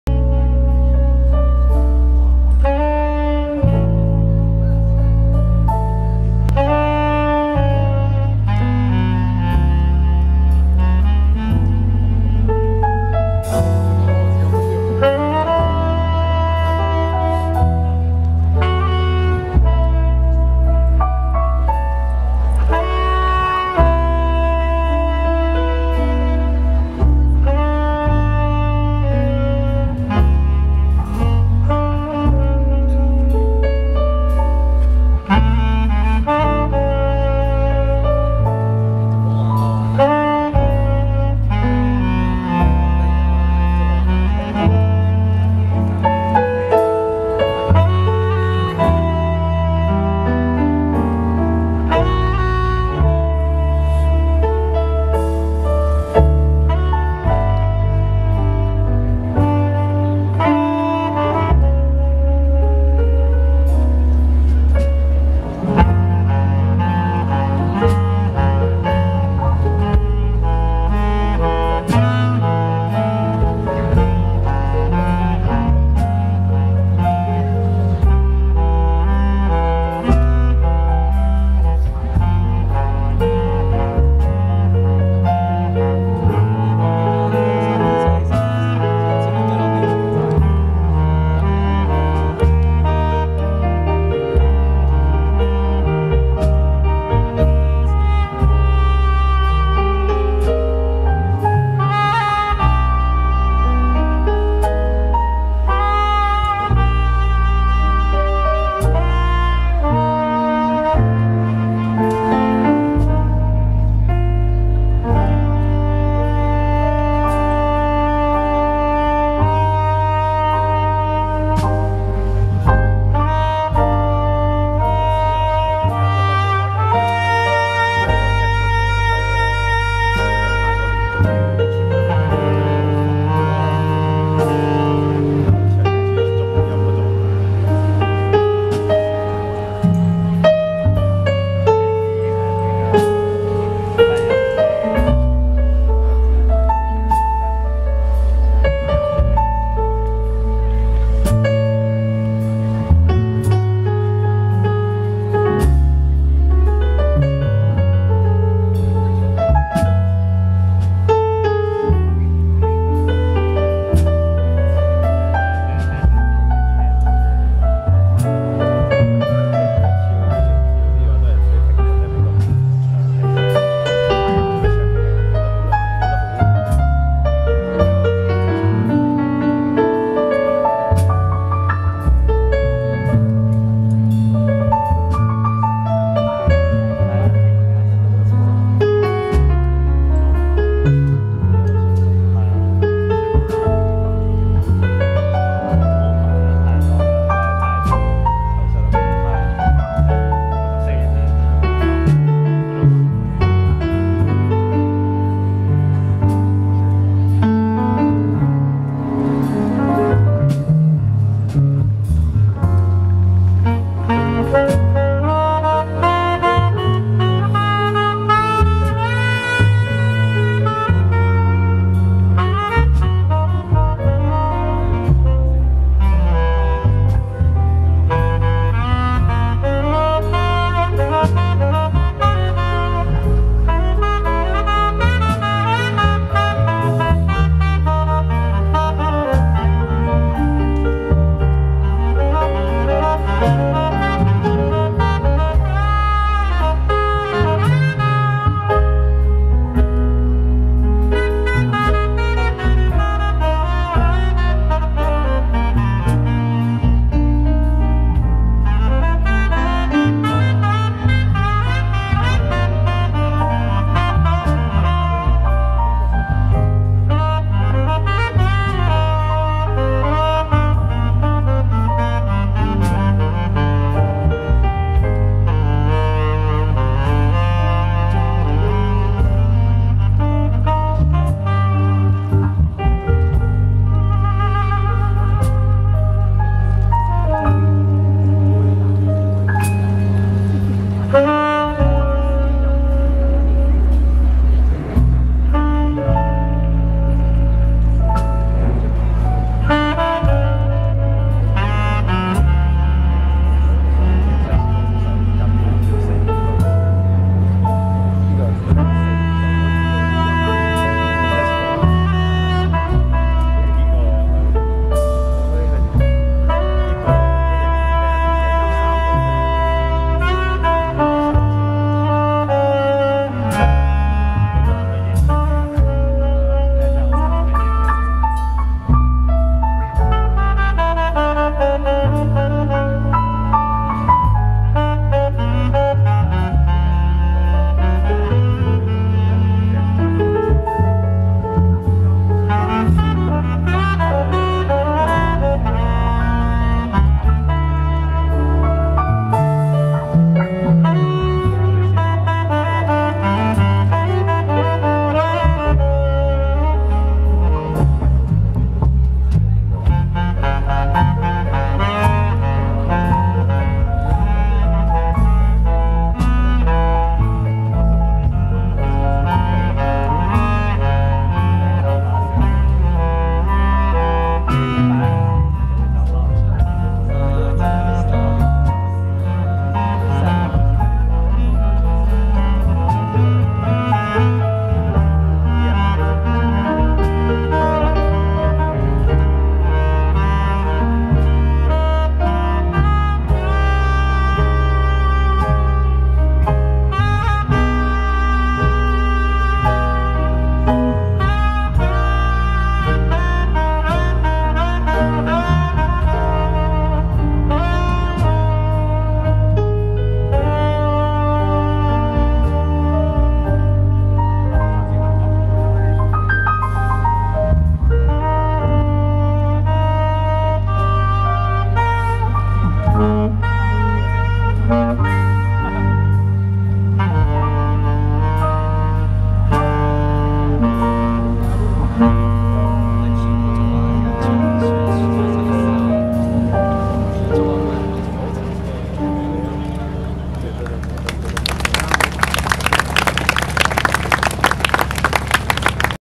Civilized culture - Jazz music (161111 DSCN0425)
{ by  @Tsimshatsui; Organizer: Hong Kong Street Music 街頭音樂 (hkstreetmusic.com); Jockey Club Street Music Series...; Relaxing, wonderful... }